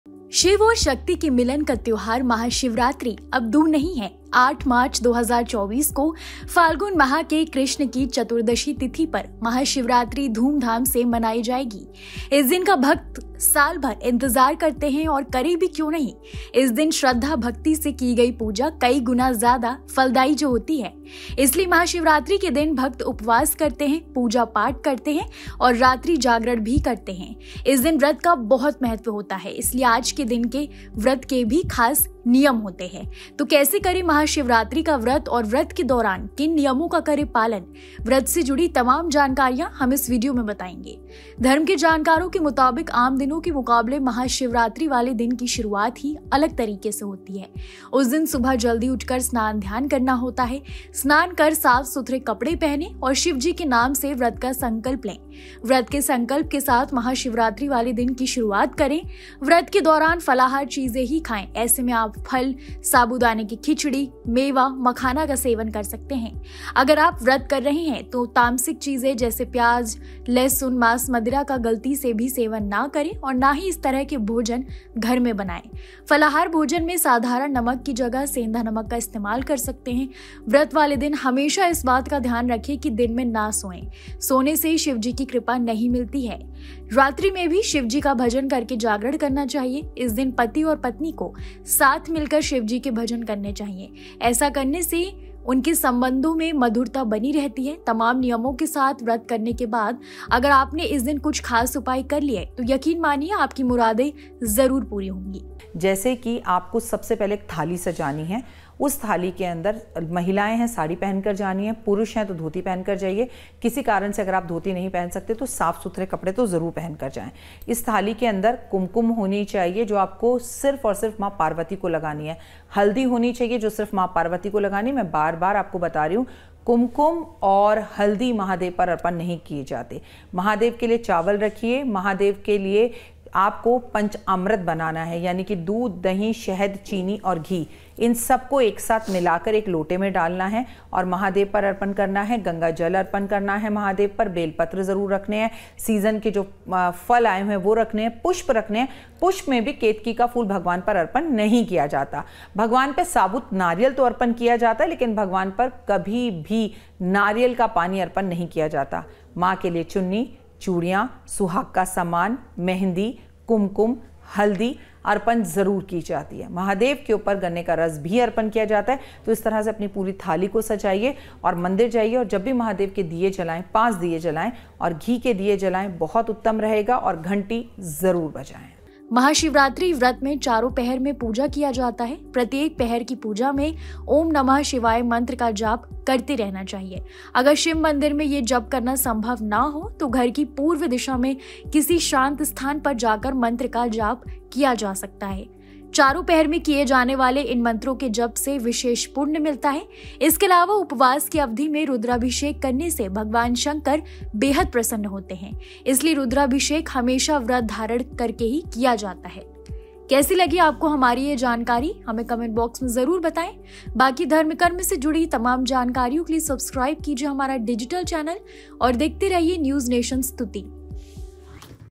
Mahashivratri 2024: शिवरात्रि पर व्रत रखने वाले इन बातों का रखें ध्यान, तभी शिव करेंगे मनोकामना पूरी
महाशिवरात्रि के दिन भक्त उपवास करते हैं पूजा पाठ करते हैं और रात्रि जागरण भी करते हैं। इस दिन व्रत का बहुत महत्व होता है इसलिए आज के दिन के व्रत के भी खास नियम होते हैं। तो कैसे करें महाशिवरात्रि का व्रत और व्रत के दौरान किन नियमों का करें पालन  व्रत से जुड़ी तमाम जानकारियां हम इस वीडियो में बताएंगे।

On the day of Mahashivratri, devotees fast, perform puja and also observe night vigil. Fasting on this day has great importance, hence there are special rules for fasting today. So how to fast on Mahashivratri and what rules should be followed during the fast, we will tell you all the information related to the fast in this video.